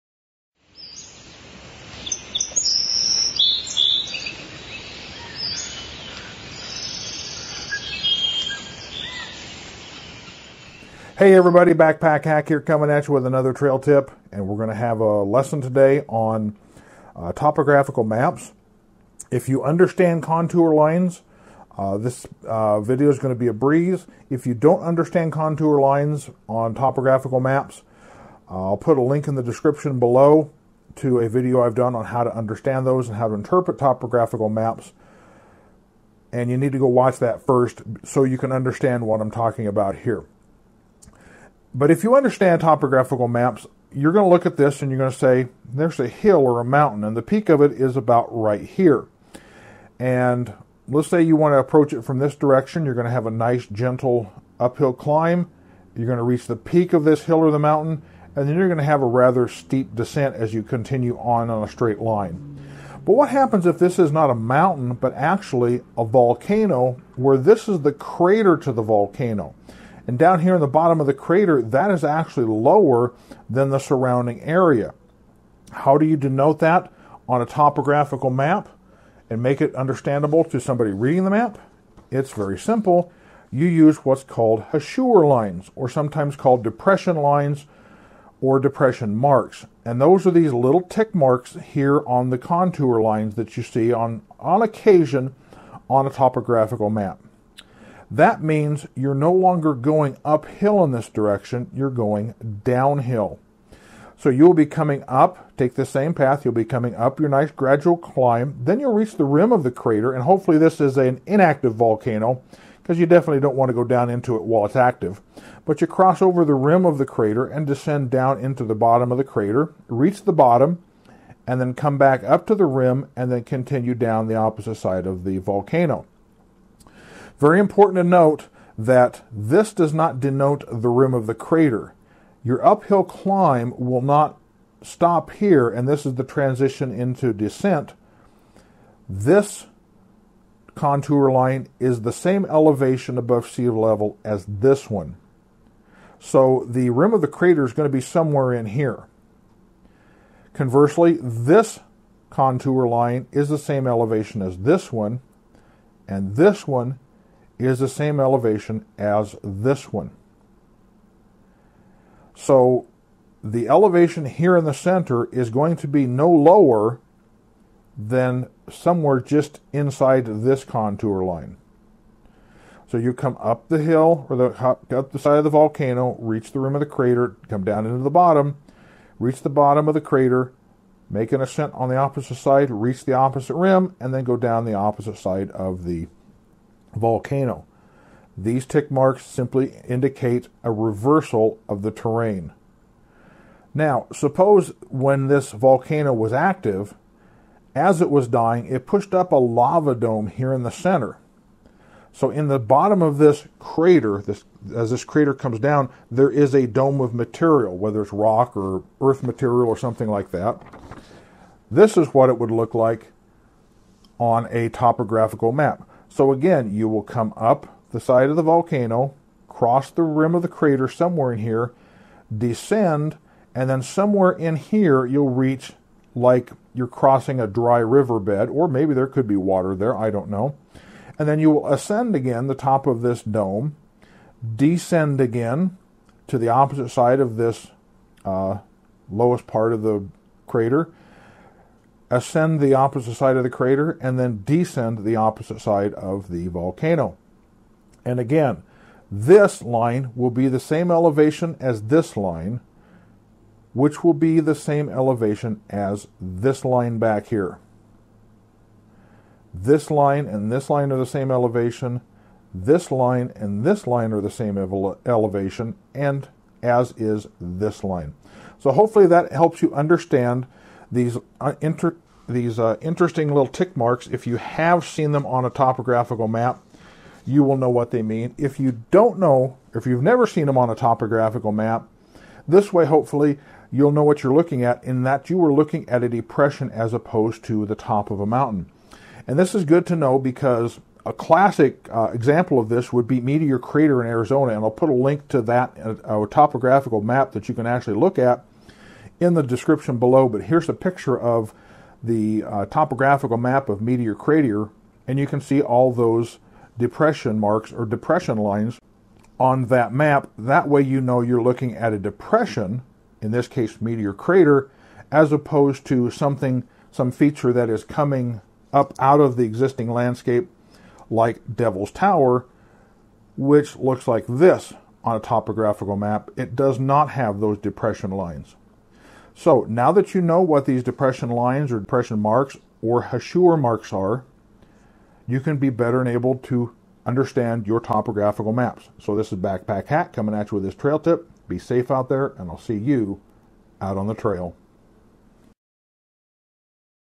Hachure Lines
Ever notice small 'tick' marks on the contour lines of a topographical map and wondered what they mean?

If you don't understand contour lines on a topographical map, check out this video: